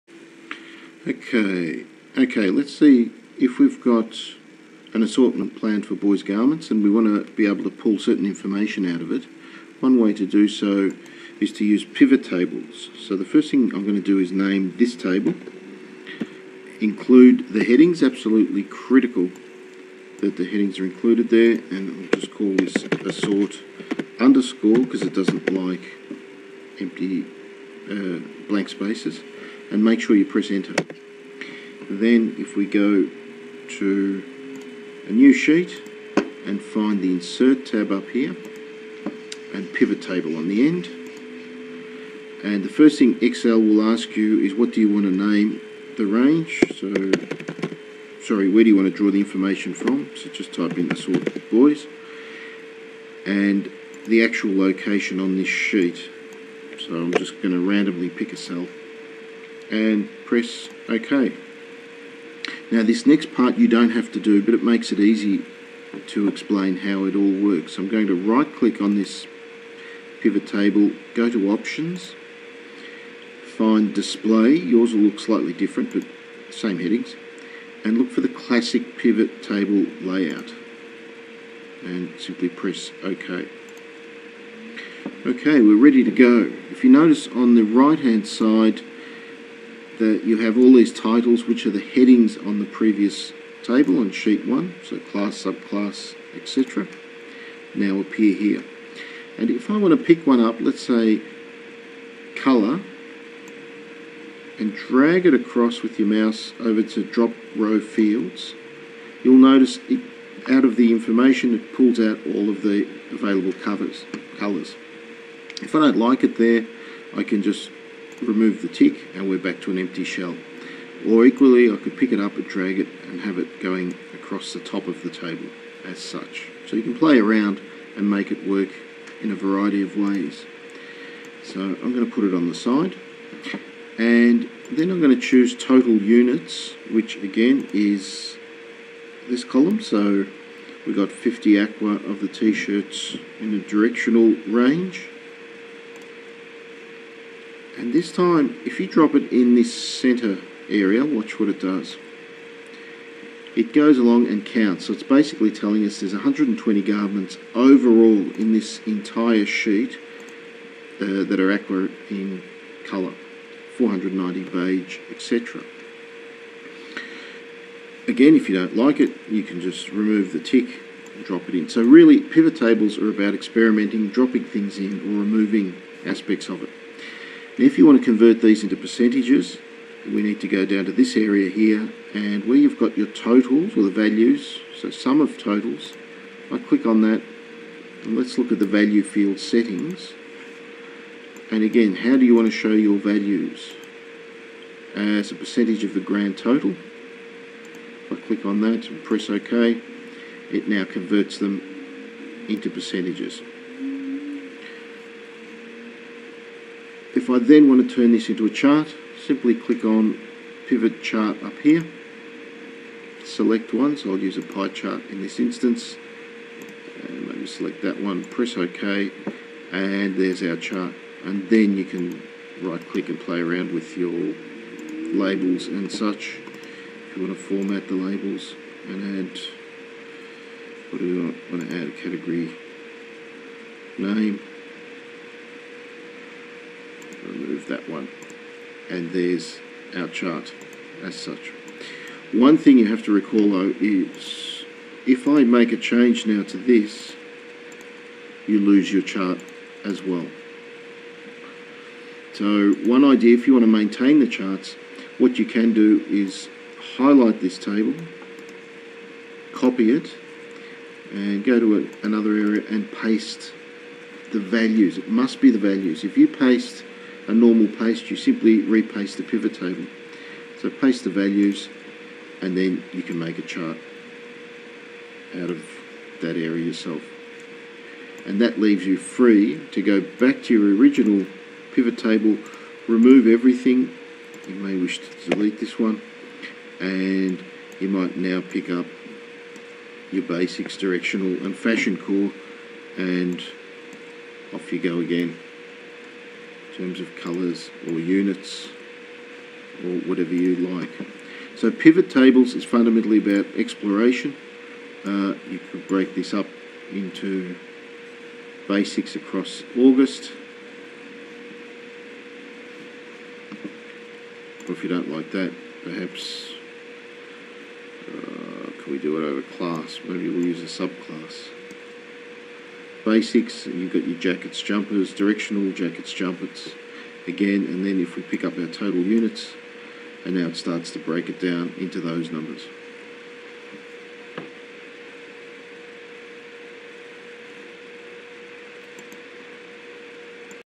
Pivot Tables Assortment Plans
Introduction to Pivot Tables and the application to Boy's clothing assortment plans